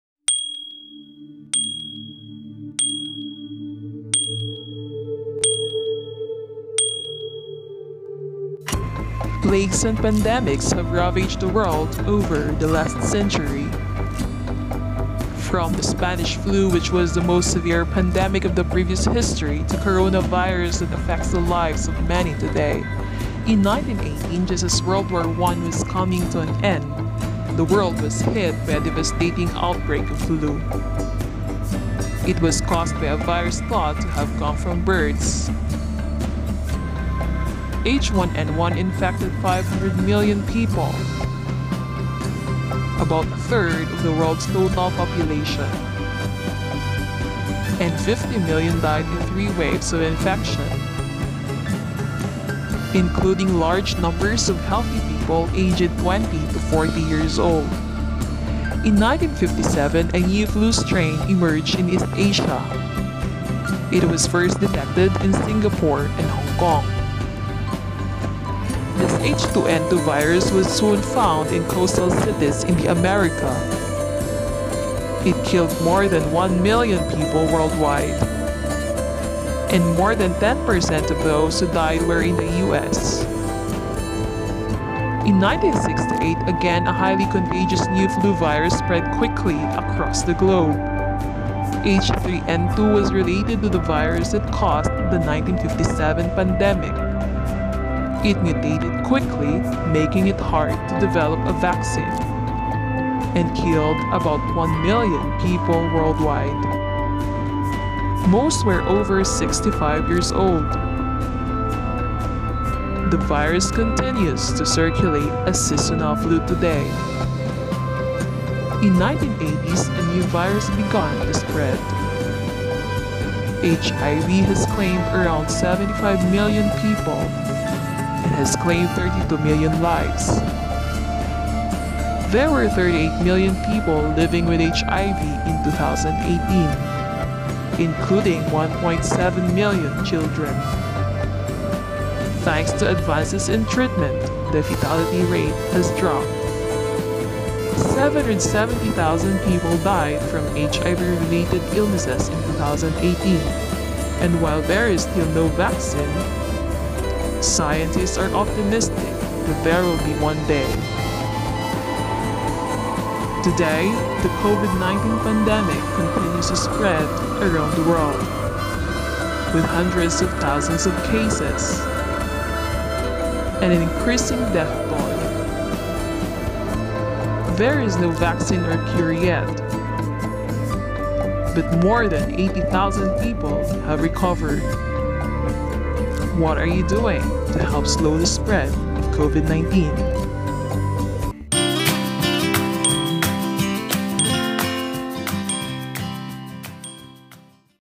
PANDEMICS in the World's History ll COVID-19 Trending
PANDEMICS have changed the world and the different people in different ways. According to WHO, "Pandemic is not a word to use lightly or carelessly. It is a word that, if misused, can cause unreasonable fear, or unjustified acceptance that the fight is over, leading to unnecessary suffering and death." Therefore, may this video becomes a realization on what and where we could do something to combat the spread of COVID-19. . .

We HEAL as One!
Let's UNITE as one!